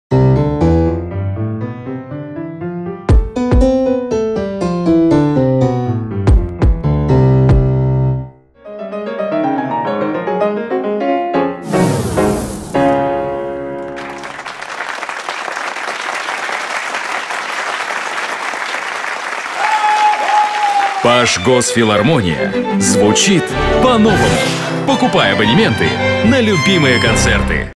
Мечта Башгосфилармония Абонементы
Проморолик "Мечта". Абонементы 77-го концертного сезона Башкирской государственной филармонии на сезон 2015/2016. Старт продаж - 16 мая 2015